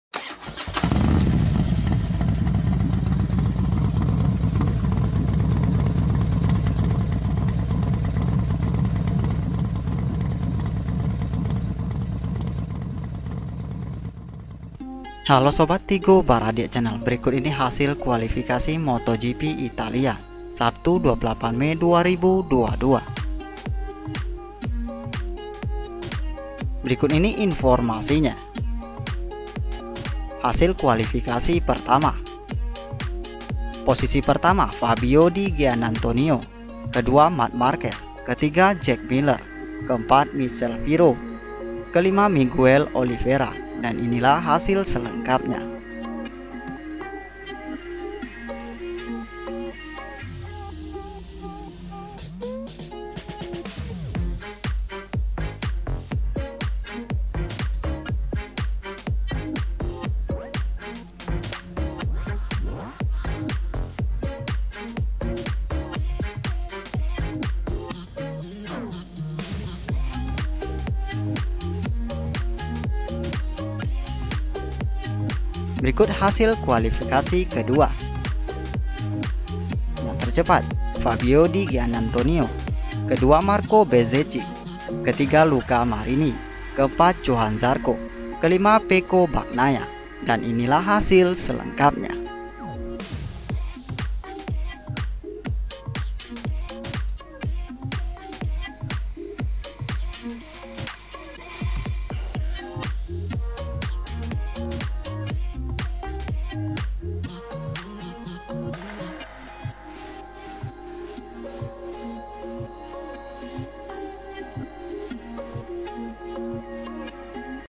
Hasil Kualifikasi MotoGP Italia 2022 ~ Fabio Di Giannantonio POLE POSITION ~ MotoGP Hari ini
Halo sobat youtube kali ini kami hadirkan lagi informasi mengenai

tigobaradiak[@]gmail[.]com